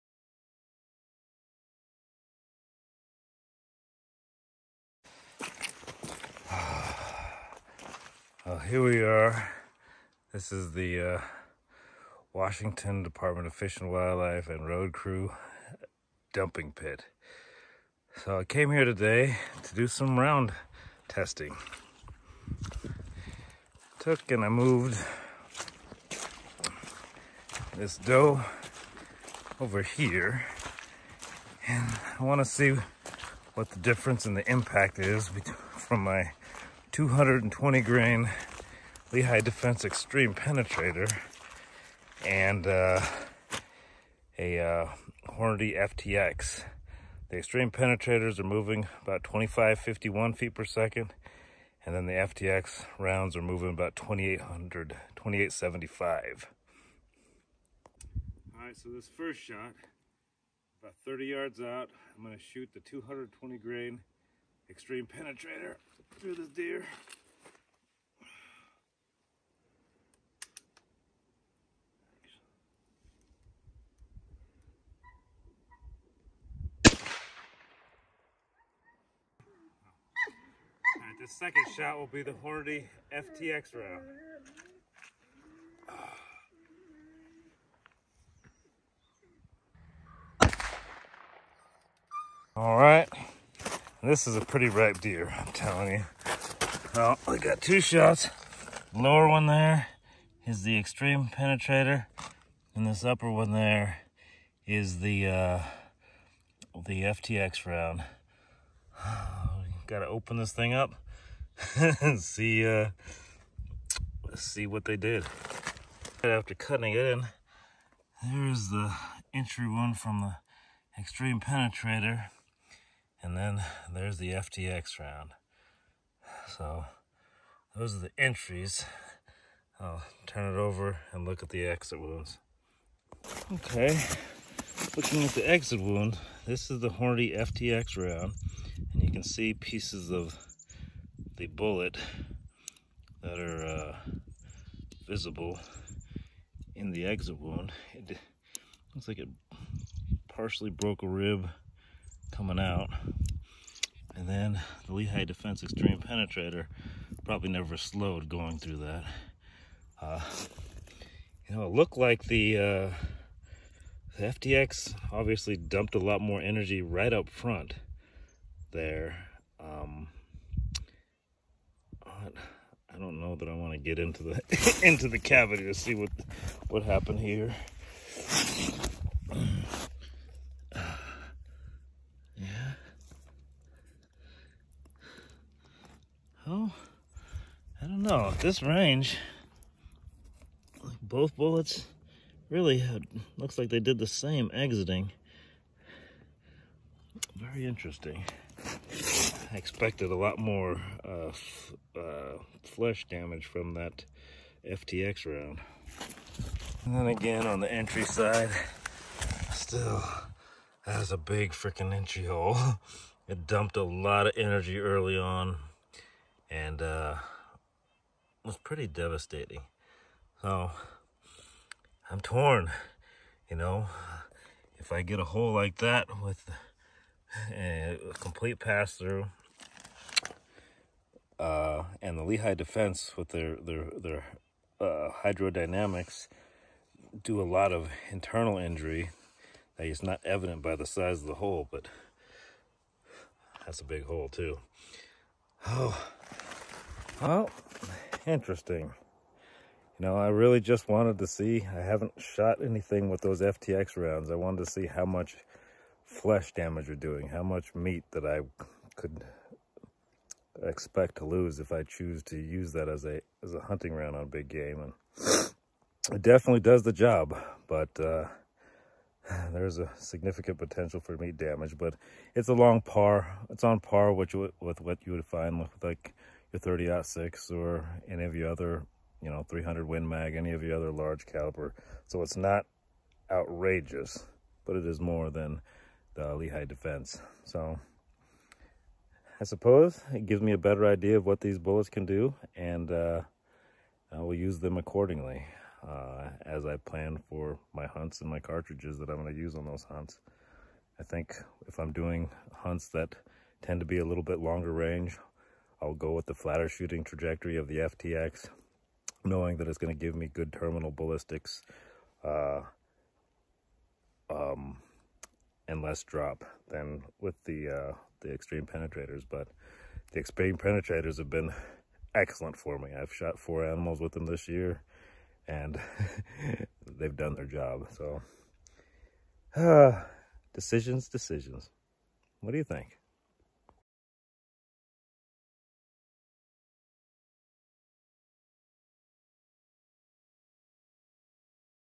Monolithic, Fluid transfer bullet vs. expanding bullet testing (on a deer carcass).
Which is better, monolithic fluid transfer bullets or polymer tipped expanding bullets?

I’m a big fan of Lehigh Defense’s Xtreme Defense and Xtreme Penetrator bullets. I’ve taken 4 animals with them, shot out of my Big Horn Armory model 90, cambered in 460 S&W Magnum and they’ve been very impressive.

Recent testing with the FTX rounds from Hornady made me want to take another look at them though. I have to say I was impressed.

Take a look at my terminal ballistic testing here and tell me what you think!